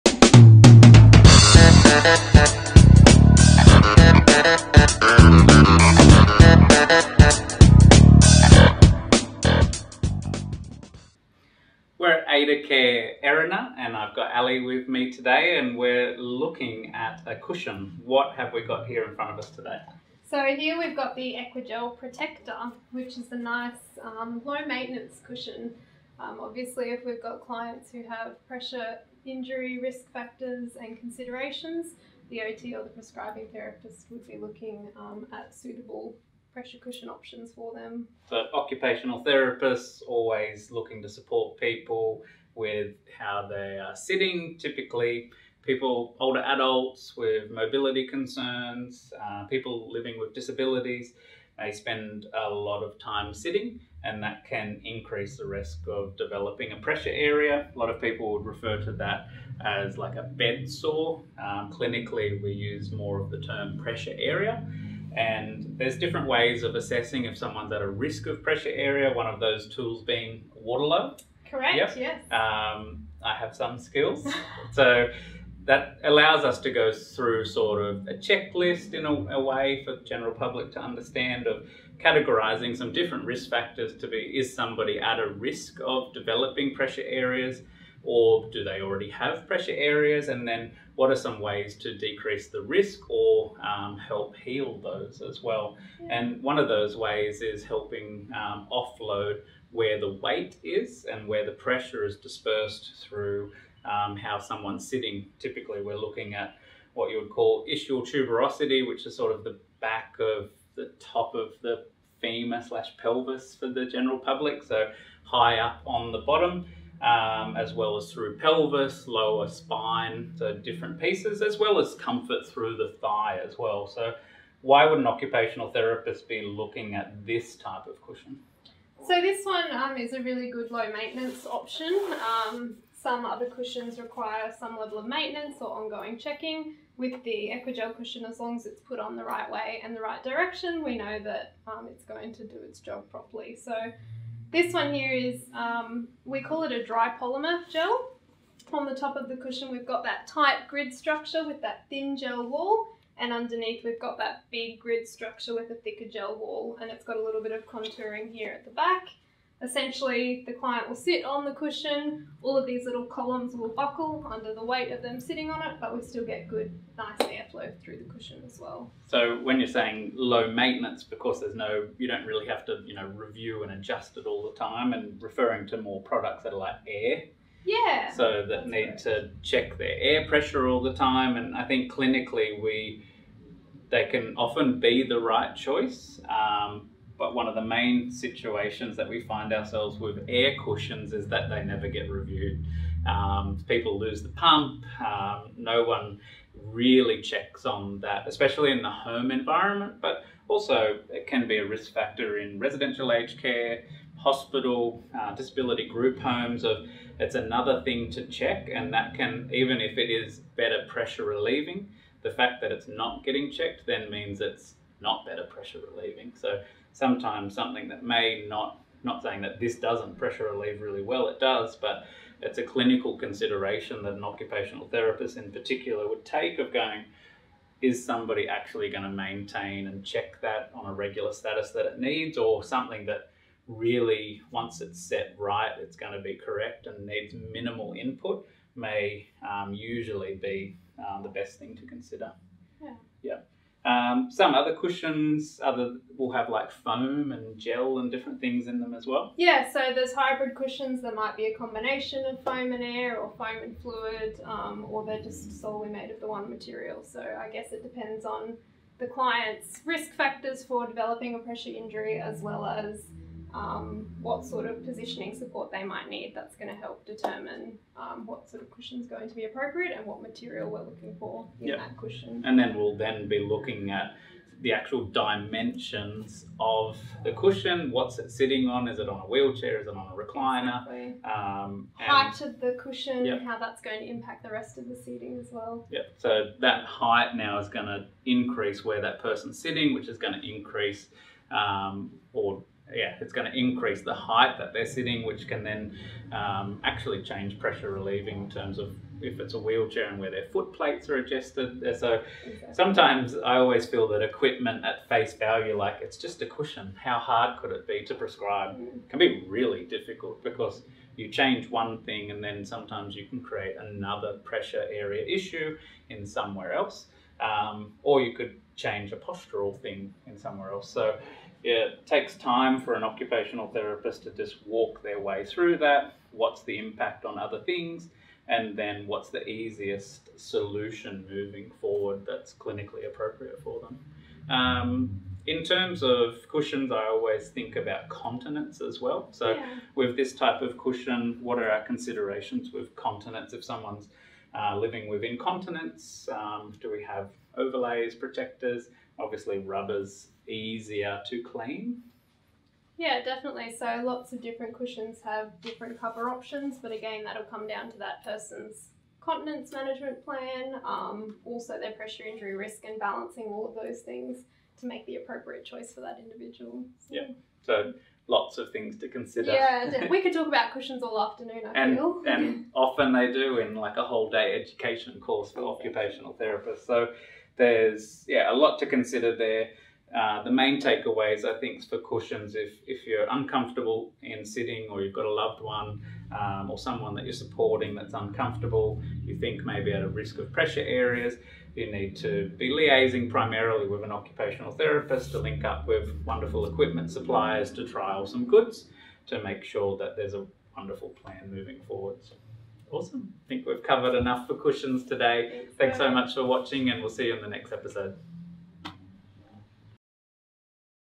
EquaGel Cushion for Pressure Care | Community Health TV - Ep 21
Dry-polymer gel cushions, such as the EquaGel Protector, don't absorb moisture and can allow air flow, which reduces the risk of pressure injuries as a result of microclimate factors.

In this episode, Scott and Ally, from Aidacare, discuss some of the main features of this fantastic, maintenance-free cushion!